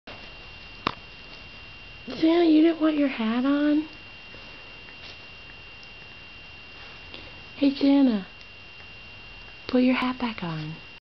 Scout & his Santa Hat
Scout got a Santa hat & cape for Christmas.